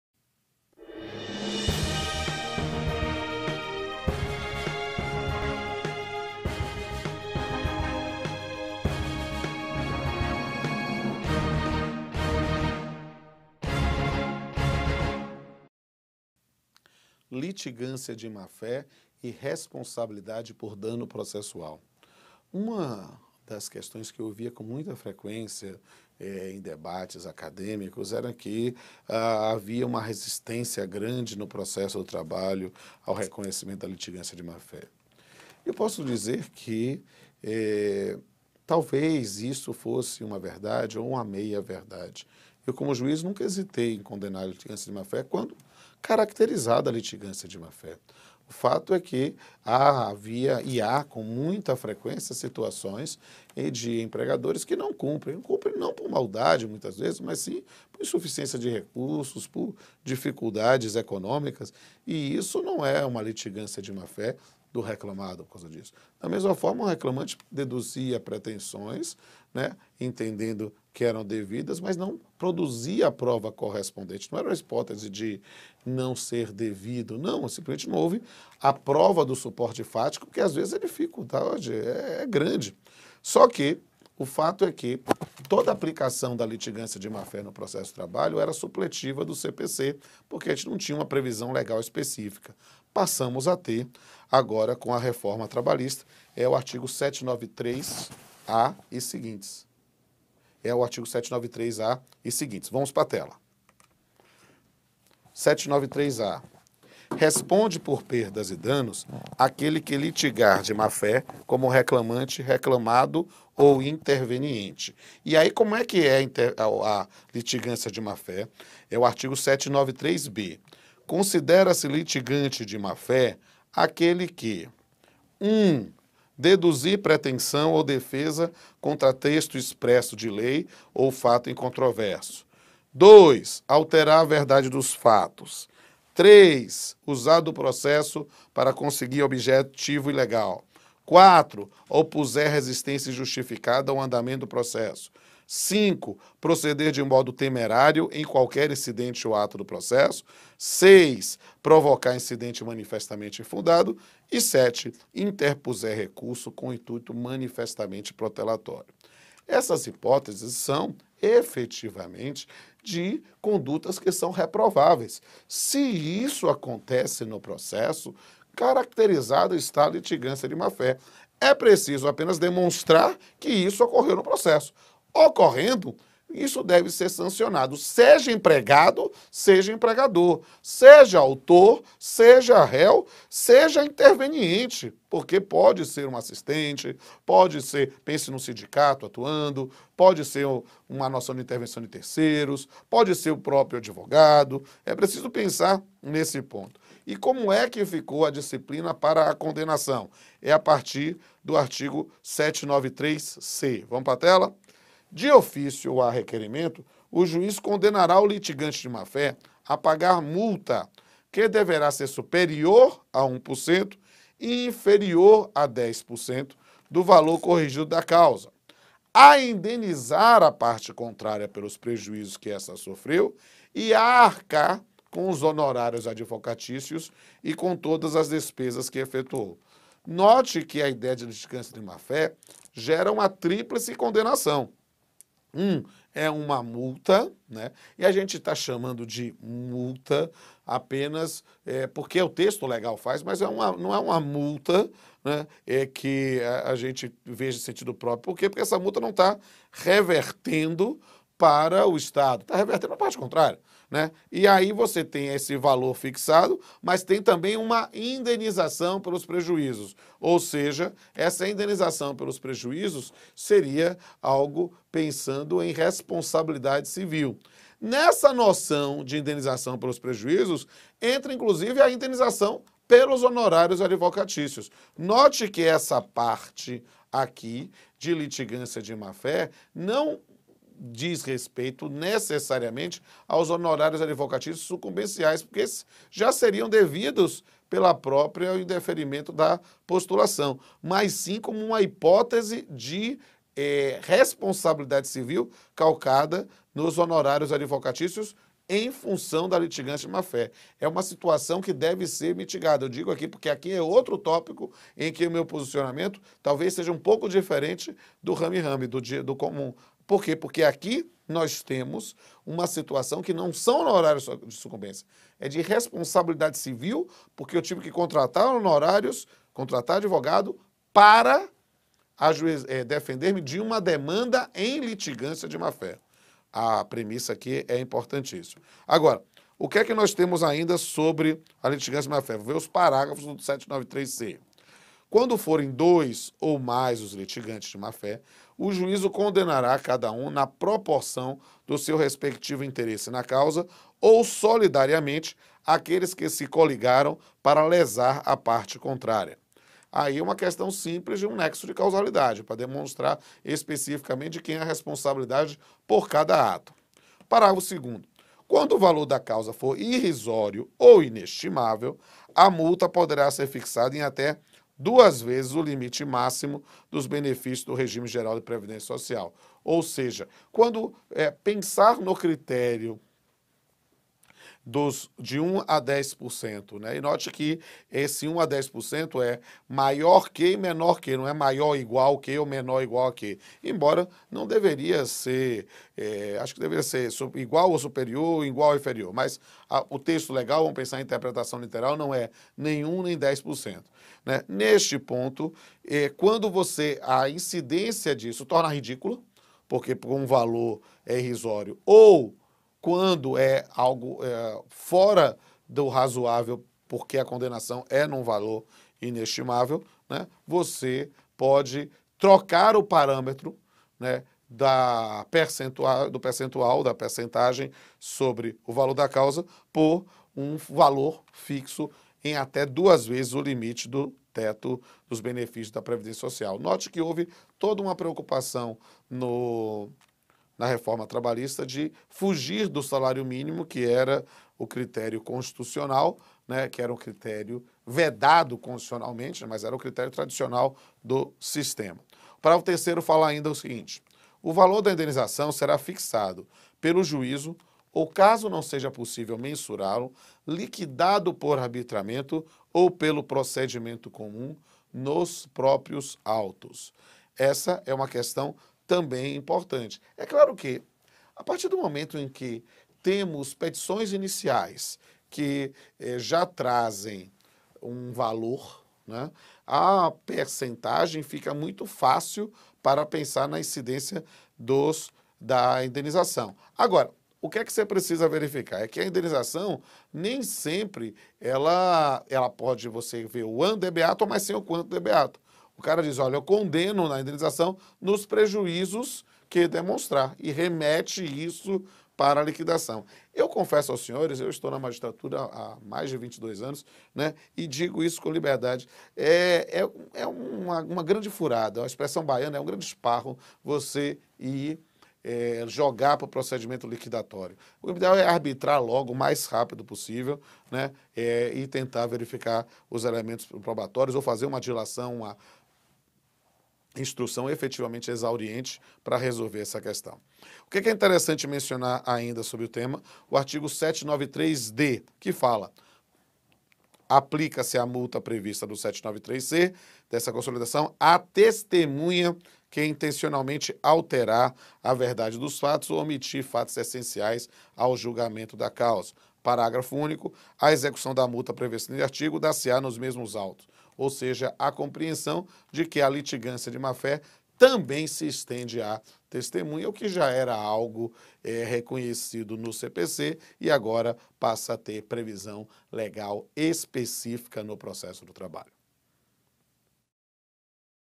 Pós Trabalho Litigância de Má Fé e Responsabilidade Processual - Vídeo 45
Nesta playlist especial o Juiz do Trabalho e Professor de graduação e pós-graduação de Direito Rodolfo Pamplona Filho esclarece, ponto a ponto, as mudanças na legislação. Acompanhe, compartilhe, mande suas dúvidas!